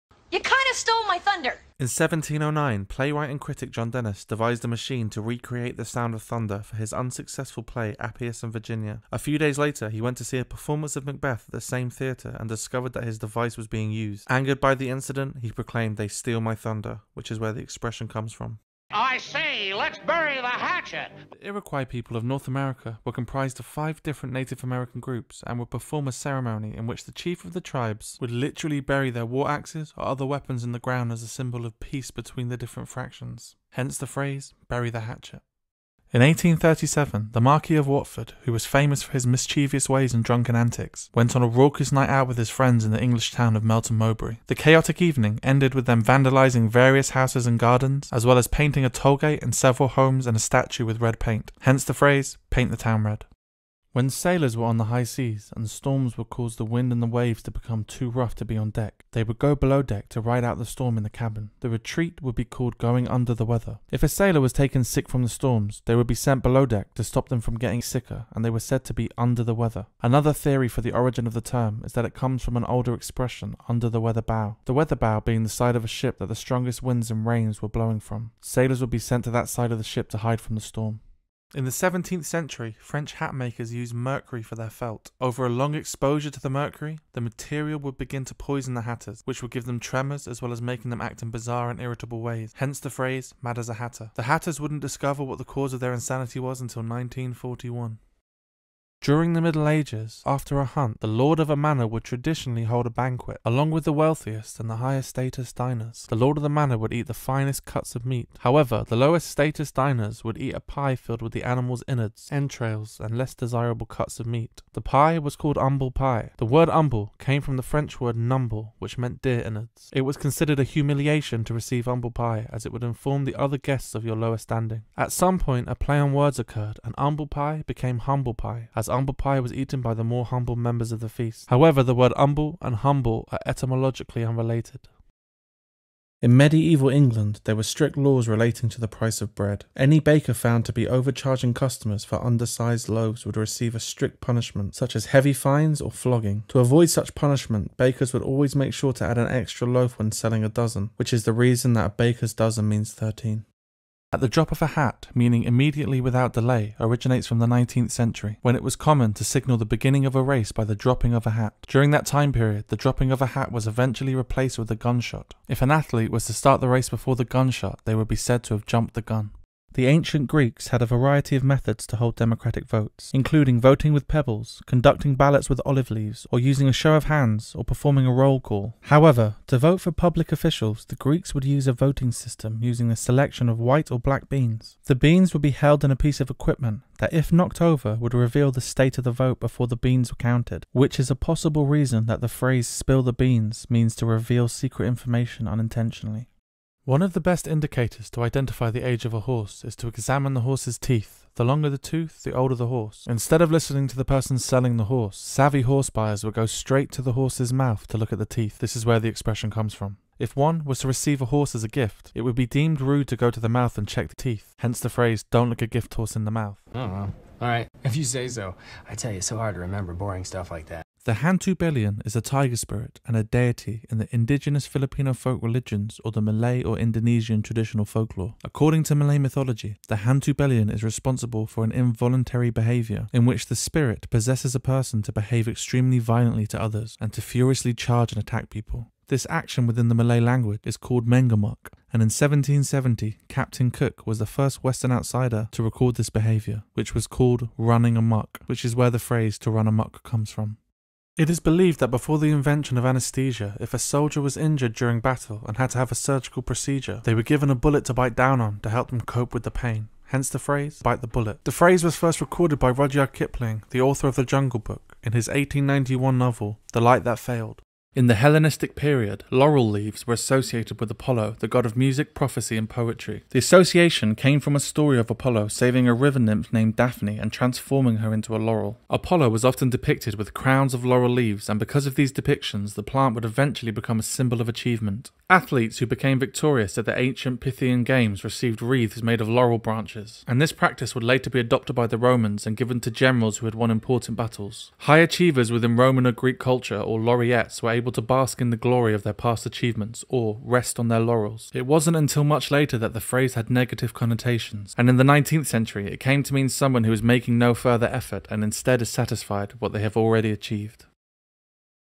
The Origins of FAMOUS Sayings
Do you know the origins of sayings such as ‘paint the town red’ or ‘humble pie', well if not, this video might be for you. The history surrounding some of the most common place phrases and sayings have a rich and surprising history. This is a 'Fascinator Shorts' collection including videos about the origins of famous sayings.

FASCINATOR YOUTUBE CHANNEL - Mini Docs on Fascinating Subjects